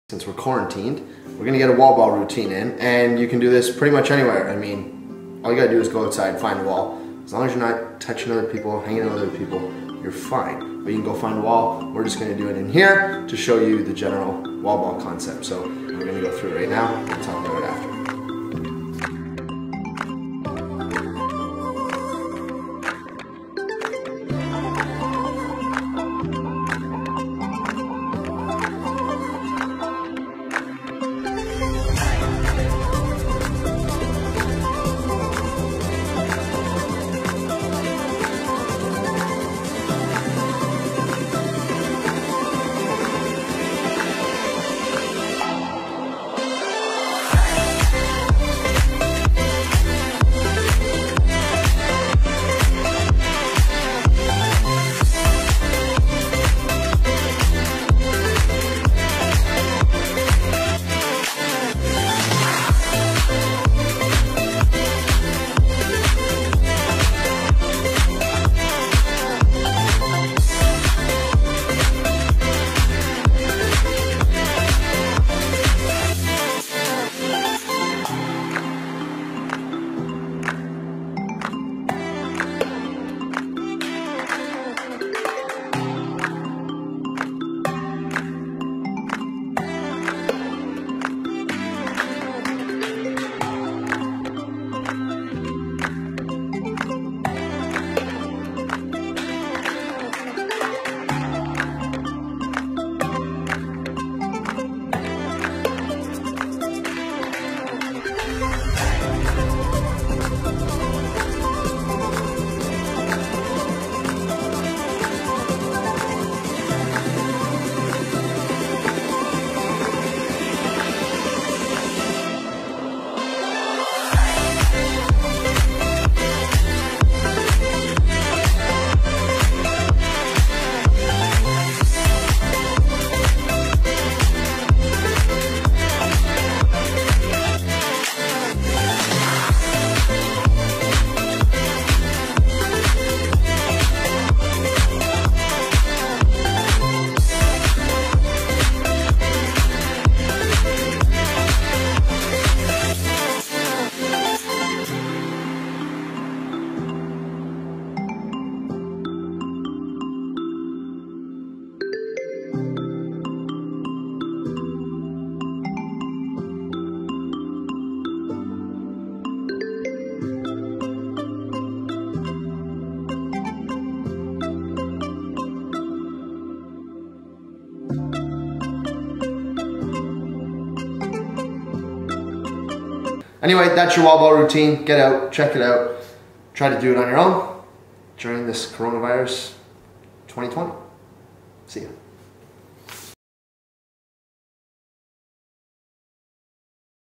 Kyle Jackson | Home Wall Ball Routine
Check out Thunderbirds forward Kyle Jackson's "Quarantined" Home Wall Ball Routine. Improve your stick skills with this routine today!